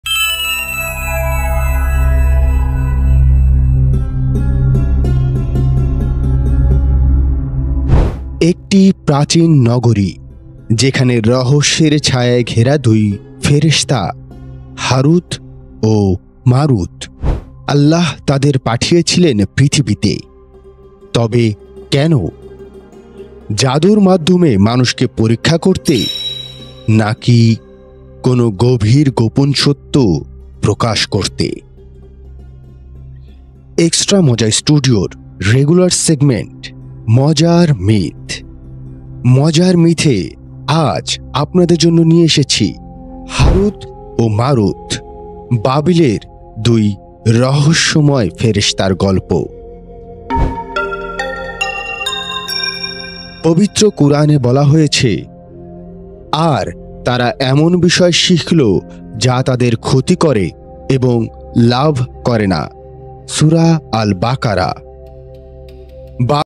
মানুষের রূপ নিয়ে পৃথিবীতে এসেছিলেন দুই ফেরেশতা, কী ছিল তাদের উদ্দেশ্য? মজার মিথ । Mojaar Myth
যাদু নিয়ে পৃথিবীতে এসেছিলেন দুই ফেরেশতা, কী ছিল তাদের উদ্দেশ্য? মজার মিথ । Mojaar Myth
Feature, Voiceover & edit: Shawkat Samudra